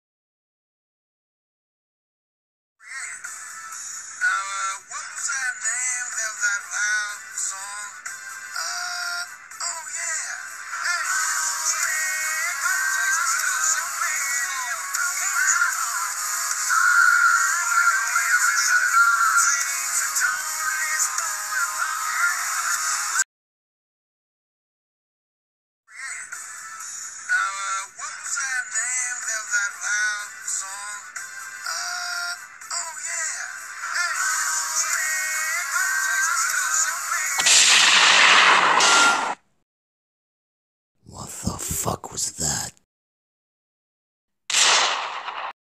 Jazz : expectations versus reality
the funniest part to me in that episode is how the Decepticons are kind of complete idiot. so I thought I'd visually represent how the scene would actually go down, God damn this writing is shit